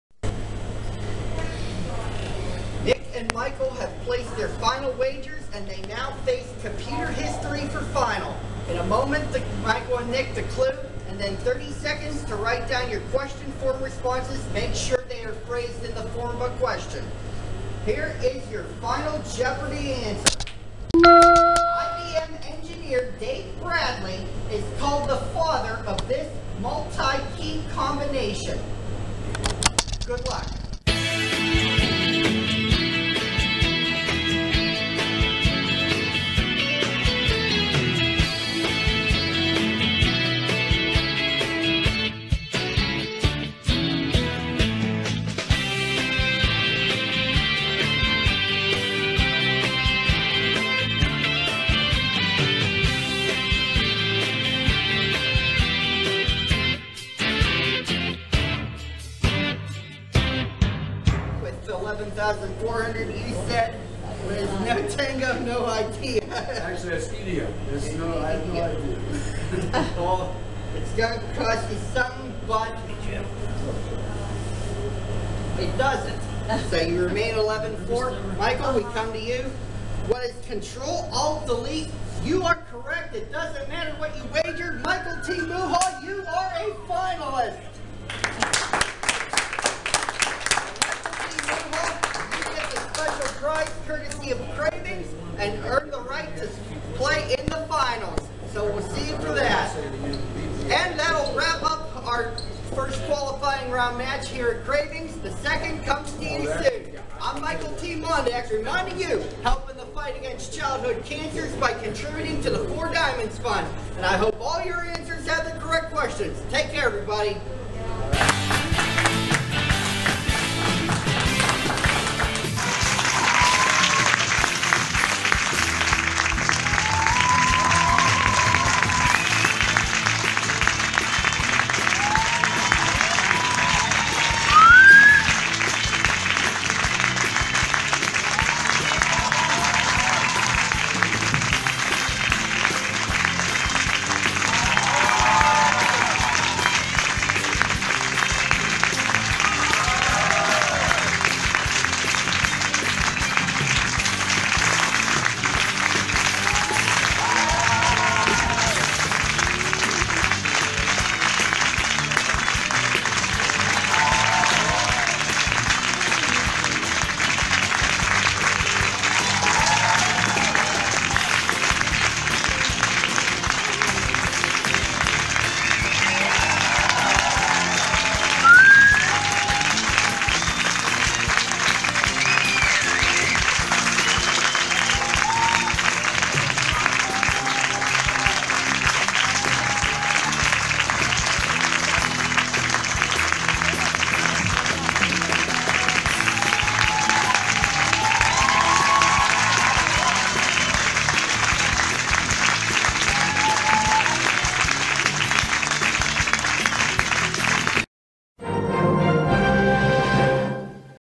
Penn State Shenango's Jeopardy! Tournament Qualifying Round Game 1 Part 4
With Adam unable to play Final Jeopardy! with no points, Nick and Michael battle in a meaningless Final due to Michael having a lock game.  Who wins?

Jeopardy! game format is a registered trademark of Jeopardy Productions, Inc.